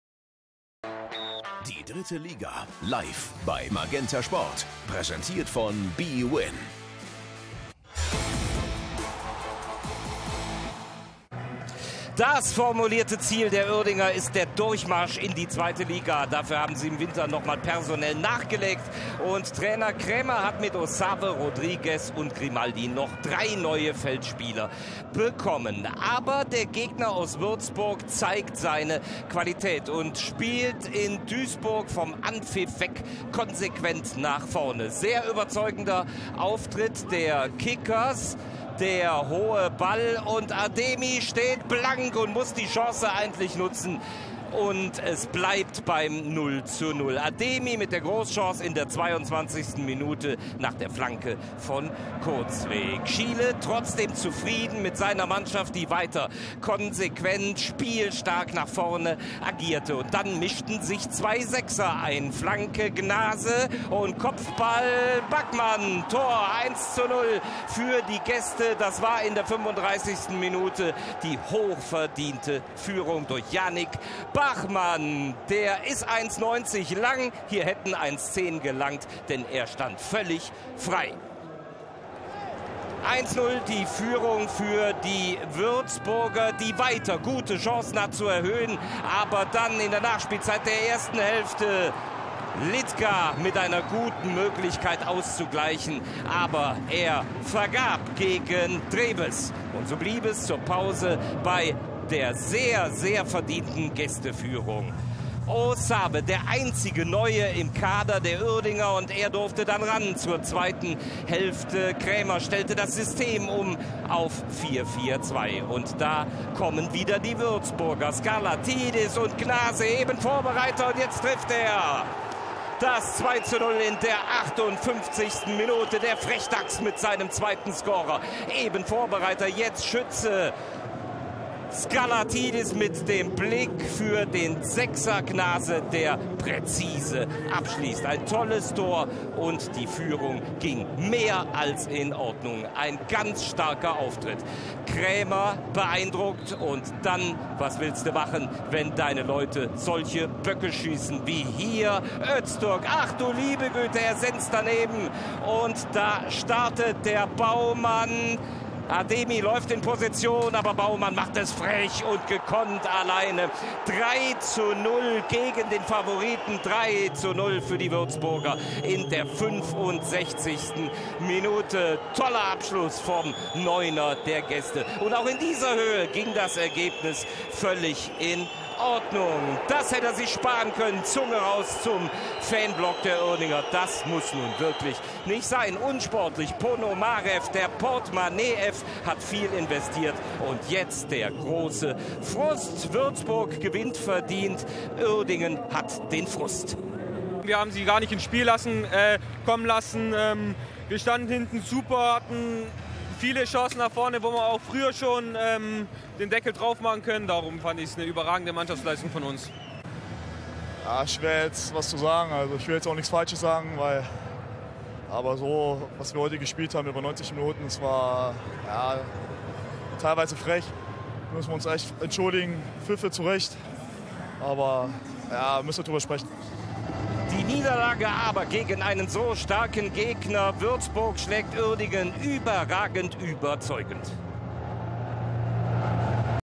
KFC Uerdingen - FC Würzburger Kickers | Spieltag 21, 18/19 | MAGENTA SPORT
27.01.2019, 21. Spieltag, 18/19. Der FC Würzburger Kickers erkämpft sich gegen den KFC Uerdingen drei Punkte!
Die Würzburger zeigten von Beginn an eine starke Vorstellung und gingen durch Janik Bachmann in Führung (35.).
Die Uerdinger zeigten sich von der engagierten Leistung beeindruckt, sodass sie keinen Zugriff aufs Spiel fanden.
In der zweiten Halbzeit ließen die Gäste nicht nach: Dave Gnaase (58.) und Dominic Baumann (65.) bauten den Vorsprung gegen den Favoriten aus.
Die Krefelder finden sich durch die 0:3-Niederlage auf dem vierten Tabellenplatz wieder.

Aufstellungen:

KFC Uerdingen:
Vollath, Großkreutz, Maroh, Schorch, Dorda, Litka (Osawe), Öztürk (Pflücke), Krempicki, Beister (Konrad), Aigner, Kefkir

FC Würzburger Kickers:
Drewes, Kurzweg, Hajtic, Hägele, Göbel, Skarlatidis, Bachmann, Gnaase (Syhre), Kaufmann, Baumann (Elva), Ademi (Breitkreuz)